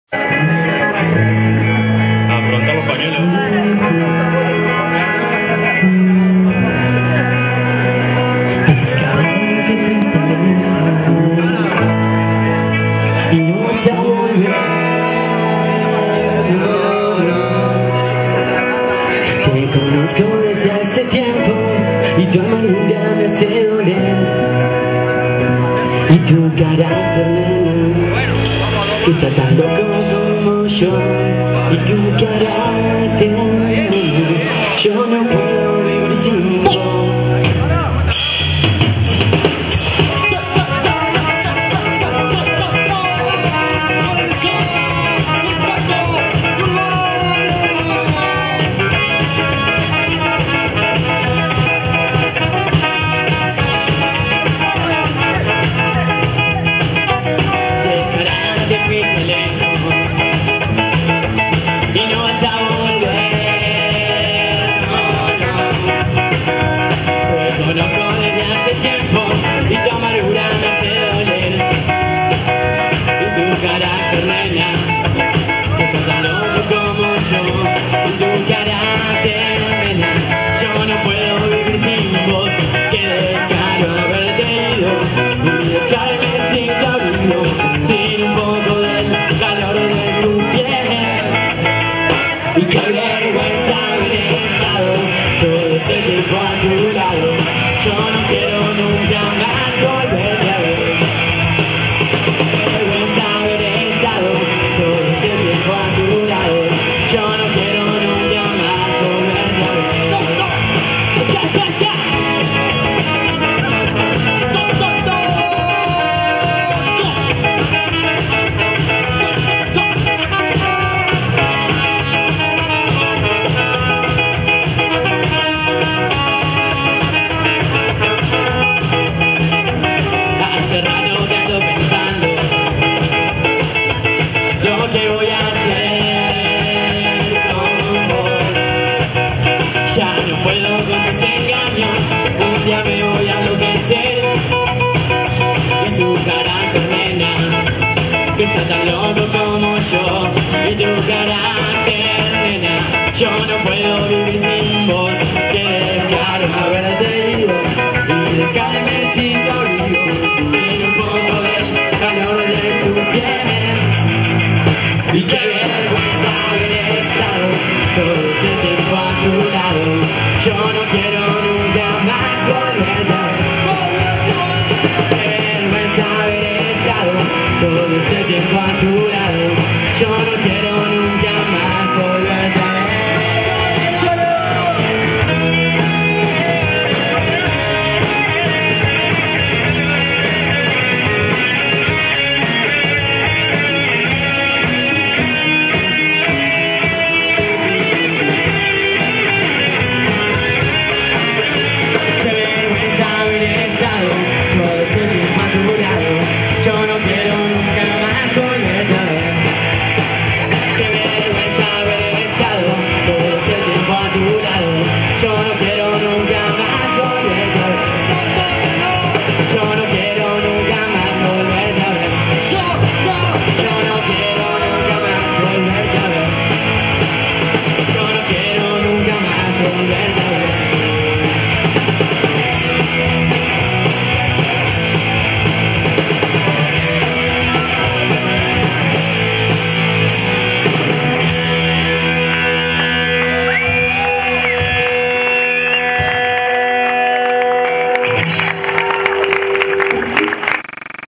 Insecto Urbano (Ska)
la banda paranaense Insecto Urbano se presentó debutando en el festival "Todo para Todos" en el Colegio Don Bosco de la capital entrerriana